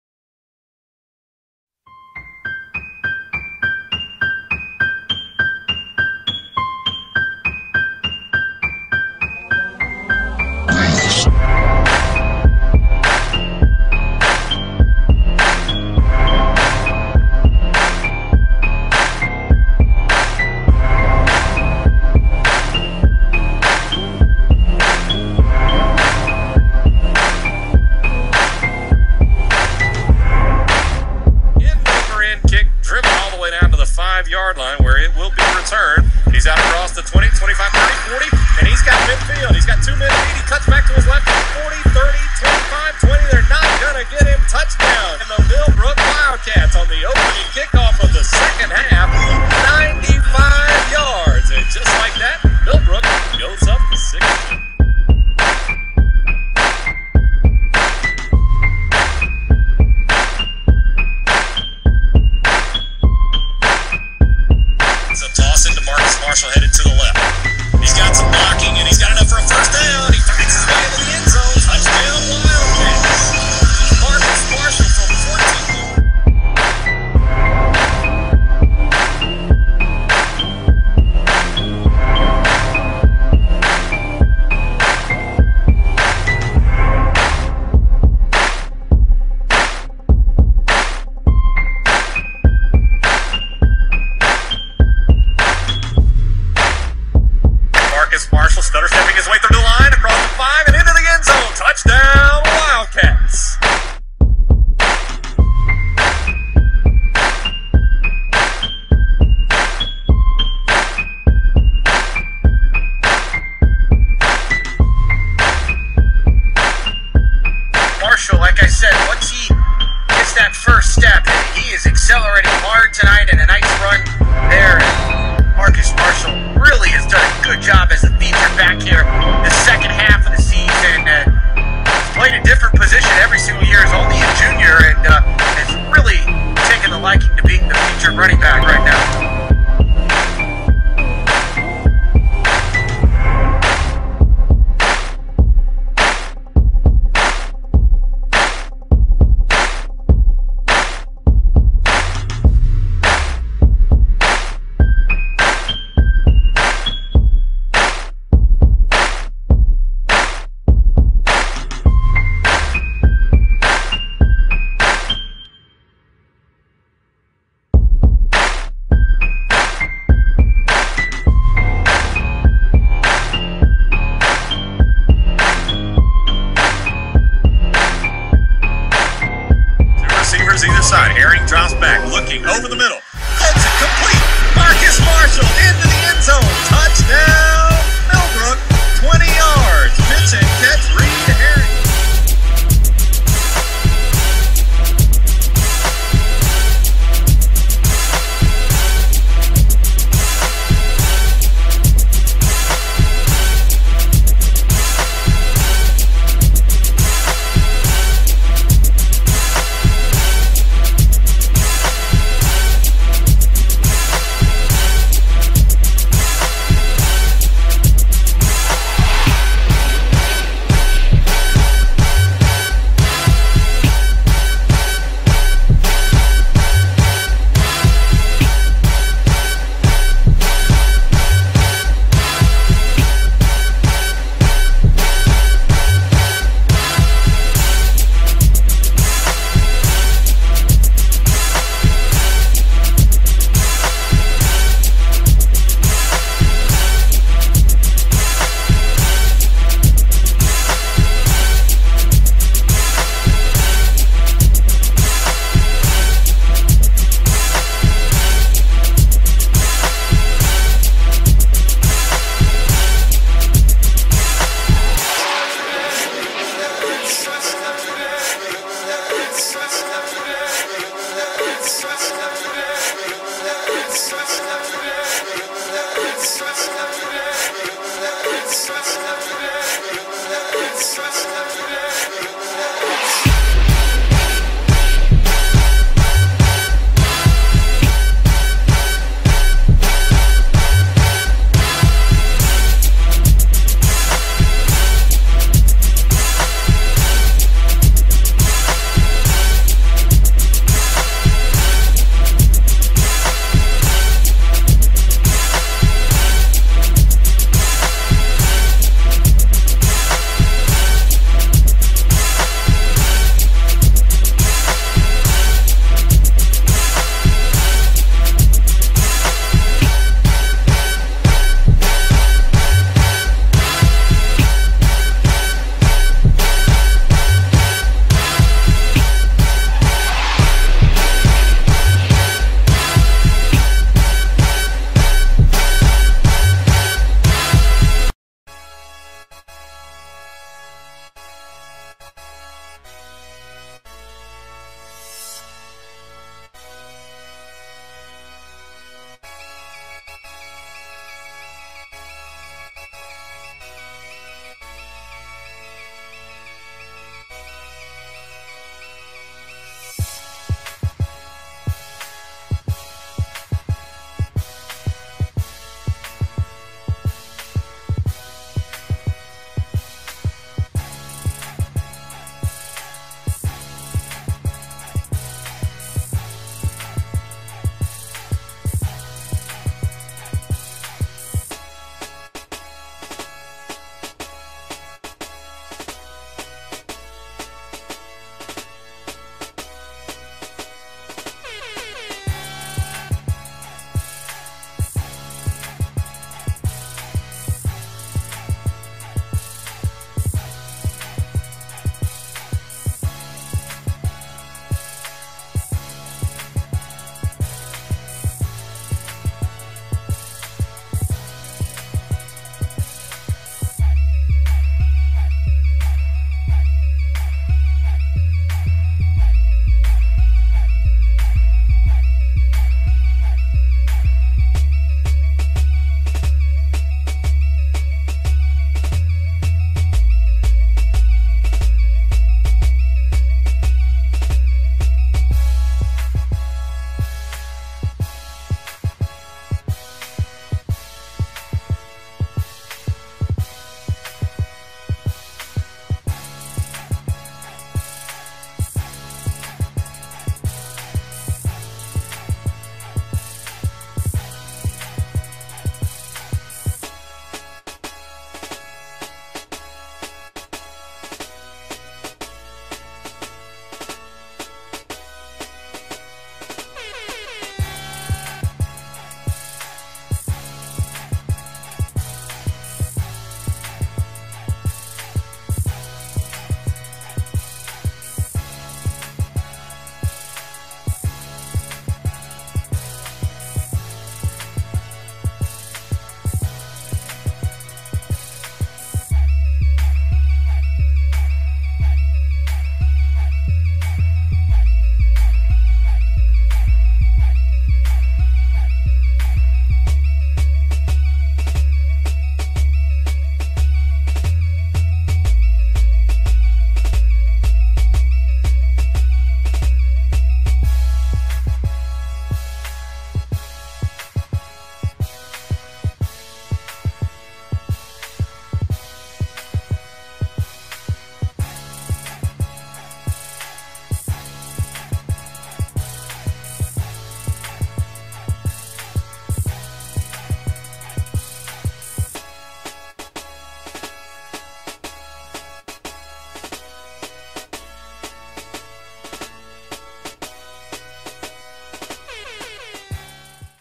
Marcus Marshall Junior Season Highlights
Marcus Marshall #21 Running Back, Wide Receiver, Defensive Back.

Class of 2015- Millbrook HS-- Raleigh, NC
4.3 GPA/1720 SAT
4.4 40 yd

2013:
1000 Rushing yards (8 TD)
300 Kick Return yards (2 TD)
250 Receiving yards (2 TD)